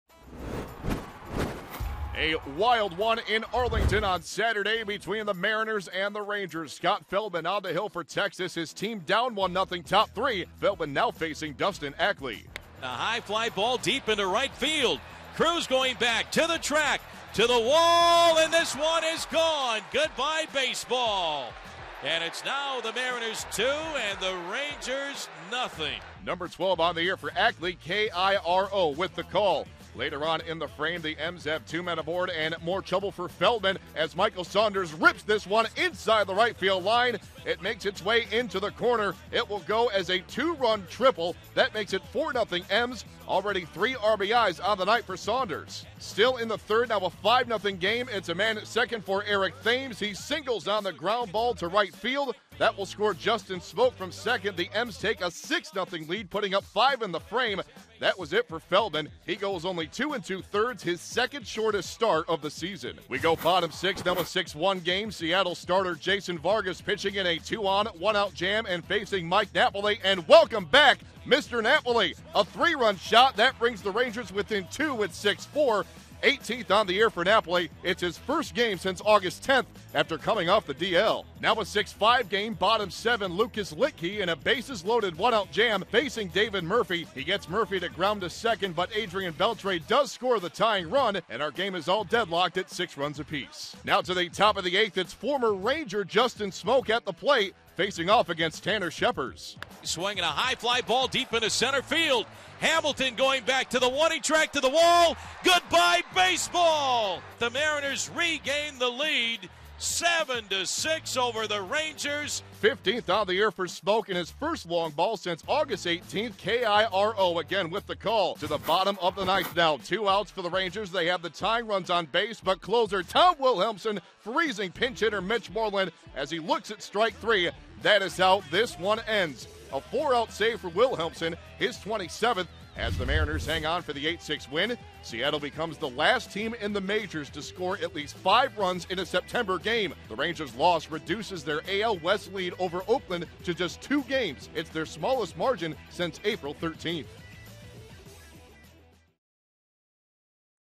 2012/09/15 Recap: SEA 8, TEX 6
9/15/12: Smoak's homer snaps tie, sparks victory